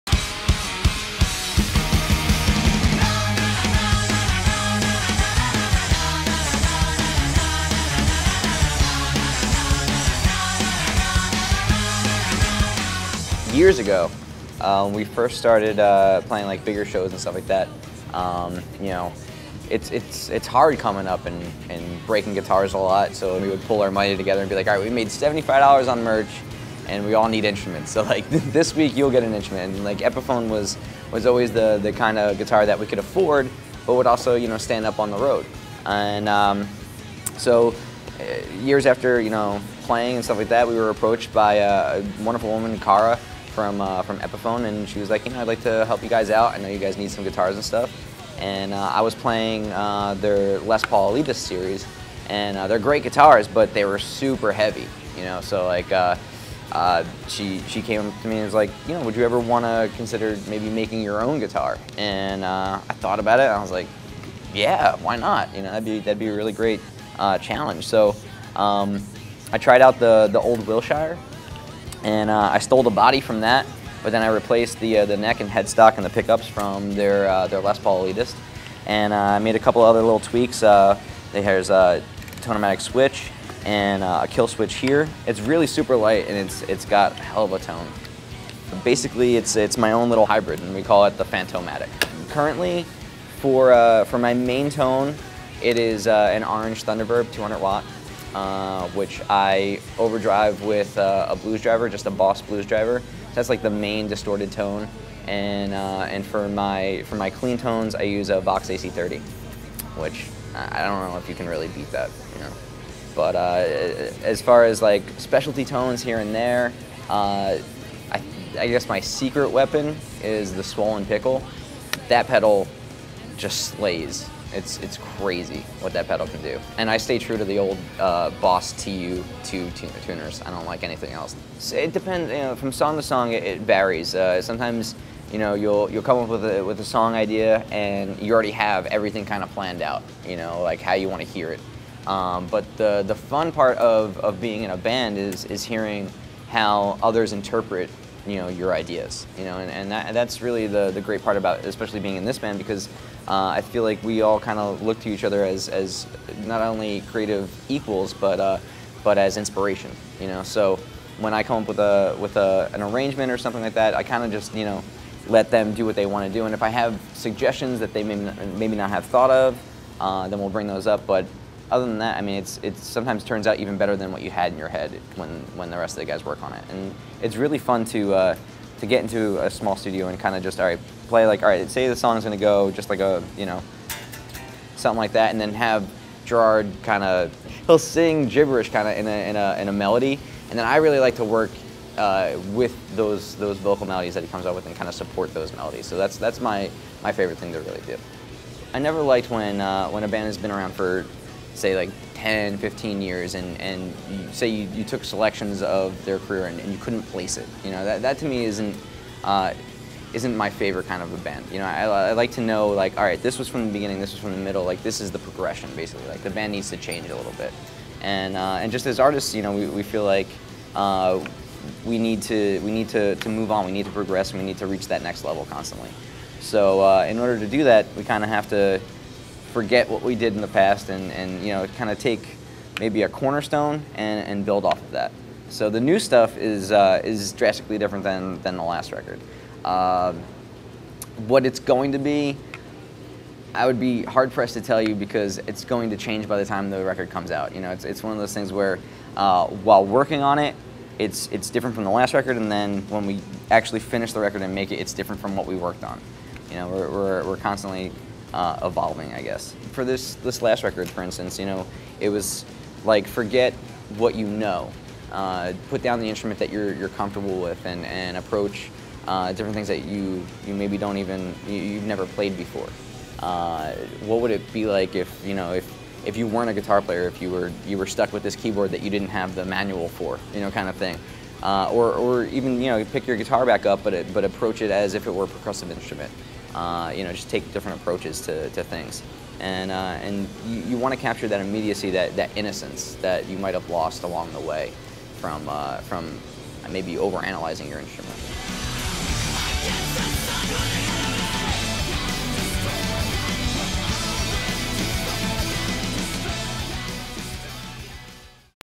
Frank Iero Interview | His Epiphone Phant-O-Matic & Other Gear | Musician's Friend
Frank Iero guitarist for My Chemical Romance talks about his gear and working with Epiphone to create the Frank Iero Signature electric guitar, an affordable, rock-solid instrument that holds up on the road. He offers a challenge to musicians, explaining how to keep your sound fresh. Frank's gear rig is now available at Musician's Friend: